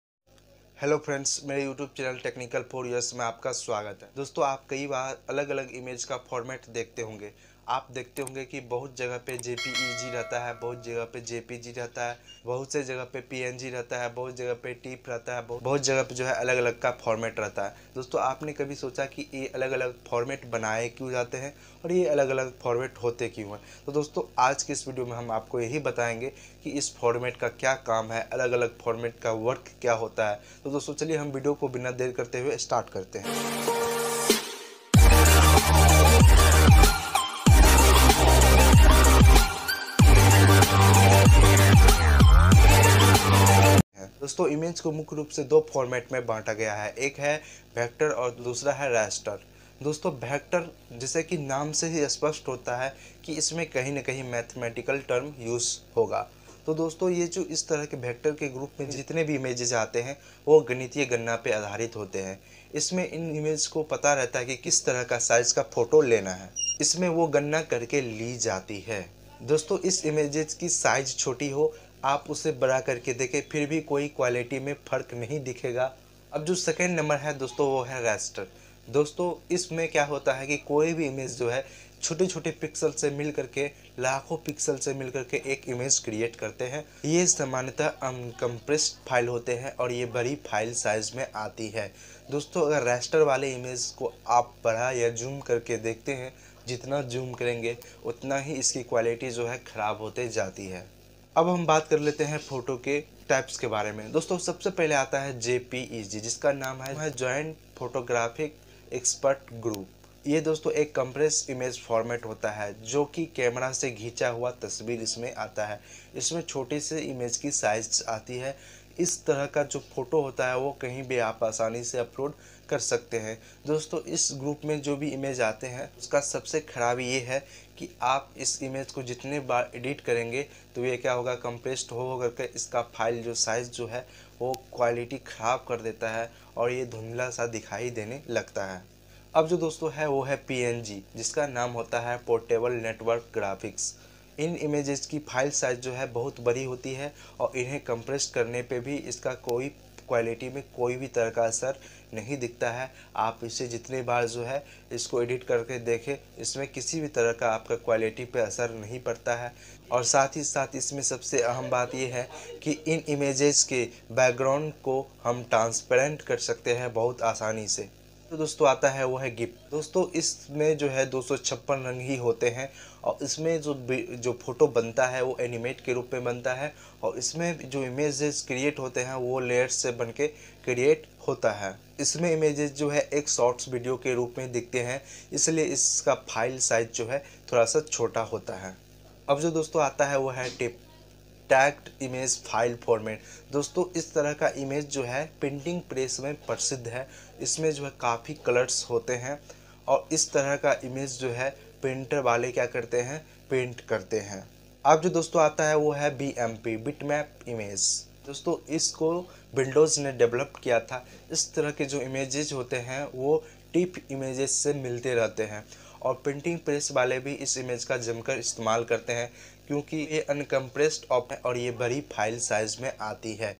image file formats explained in hindi - JPEG, PNG, GIF, TIFF, BMP etc
Dosto apne dekha hoga ki bahut bar alag alag image formats rhta hai kya aapne kabhi socha es alag alag formats ka use kya hai.

1. image ka formats
2. image alag alag formats ka kyu rahata hai.
3. png kya hai
4. jpeg kya hai
5. alag alag image formats kyu use hota hai
6. image formats defined in hindi

             jay hind